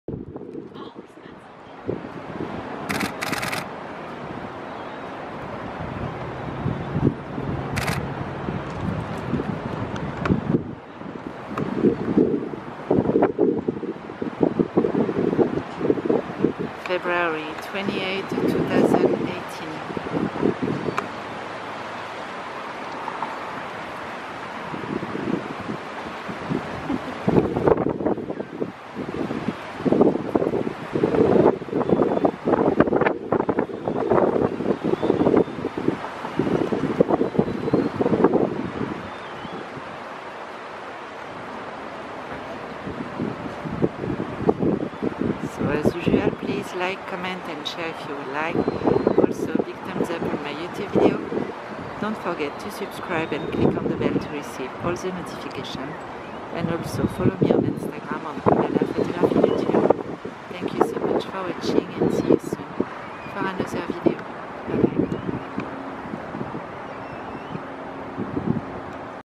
The Woodlands TX Bald Eagle - Martha and her Twins Eaglets Miracle & Hope - 02.28.2018
Martha arrives with a fish for her 2 months and 2 days old Twins Eaglets Miracle & Hope (62 days old) Sadly my camera wasn't set up correctly but you can see the fish anyway.